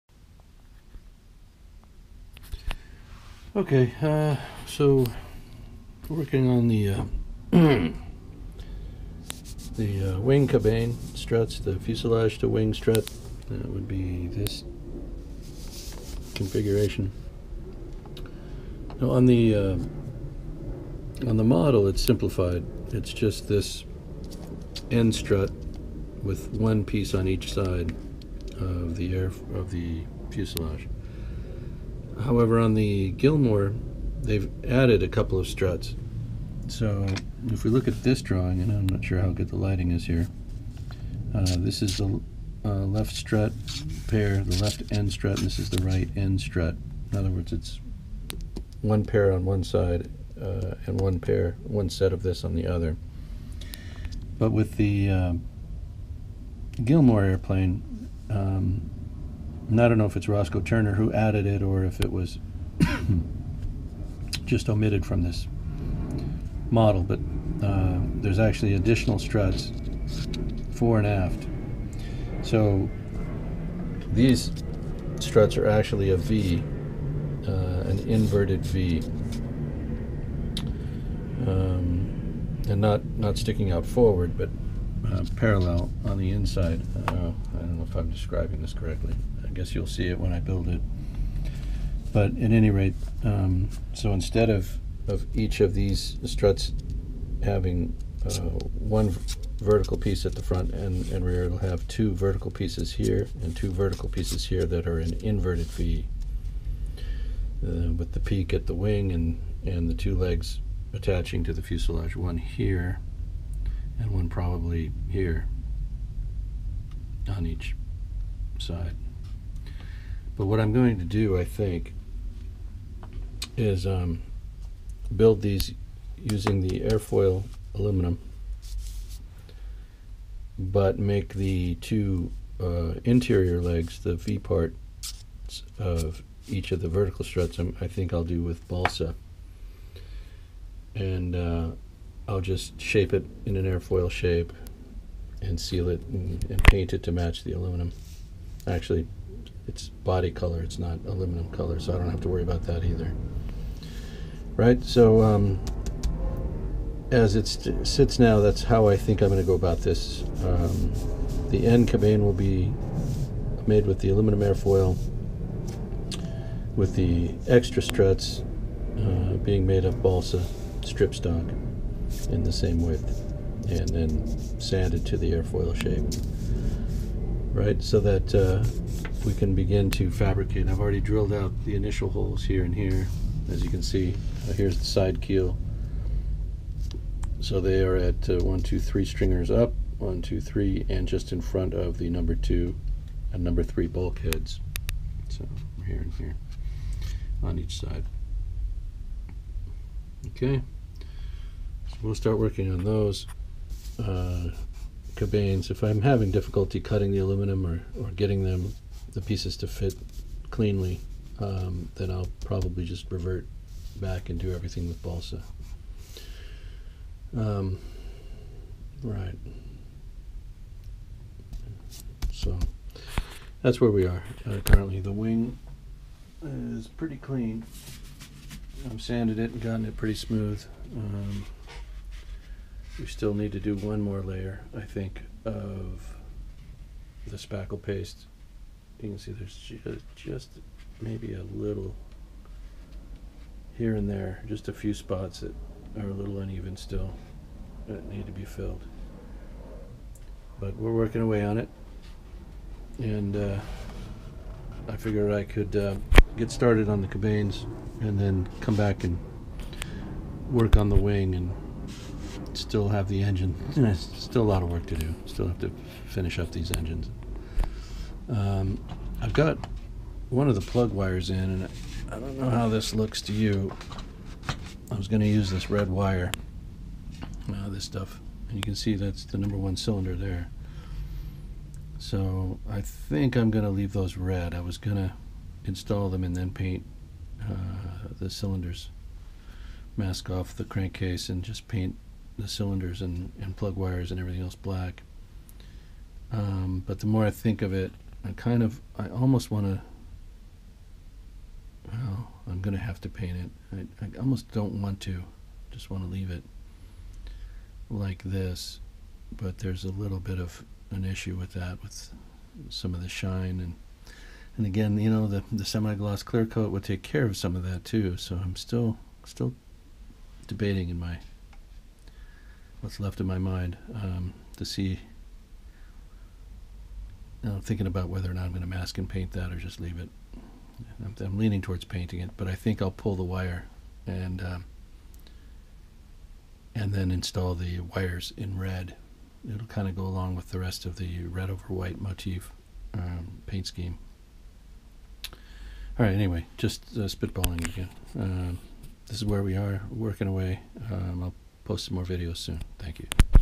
Air Express - Part 21
Looking at reference photo's of the aircraft to see exactly what the wing cabane struts should look like - as the Dumas kit drawings represent only a simplified solution meant to save weight..and is not an accurate representation (but certainly more than adequate for a rubber band flyer) for a display model.